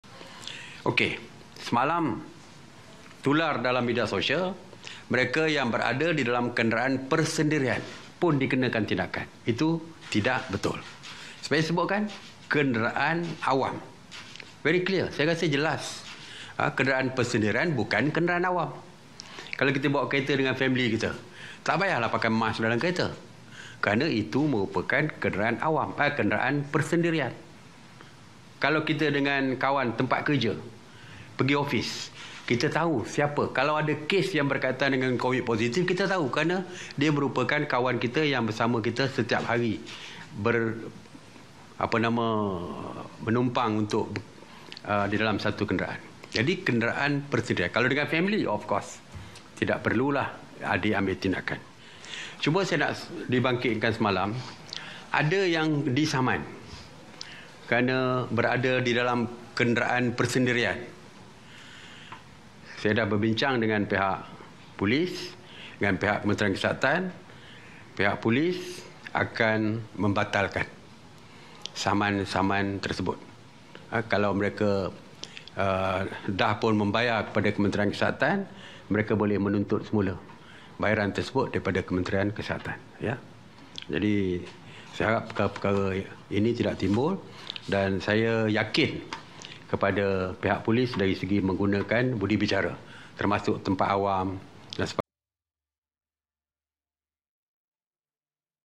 Pelitup muka: Kompaun tidak dikenakan bagi kenderaan persendirian
Saman yang dikenakan terhadap individu kerana tidak memakai pelitup muka di dalam kenderaan persendirian akan dibatalkan.